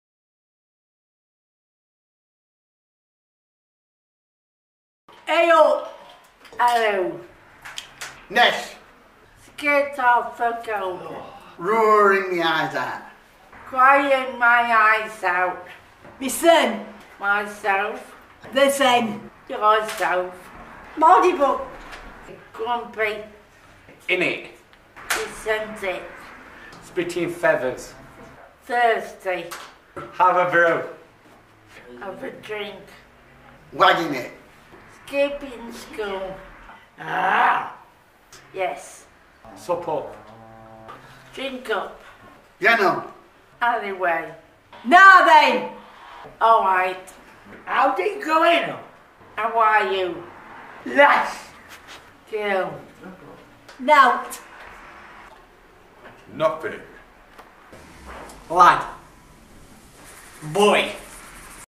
Radio Burton Street Red Nose Day Sheffield Slang
Our Radio Burton Street hosts clue you into the local lingo Sheffield style.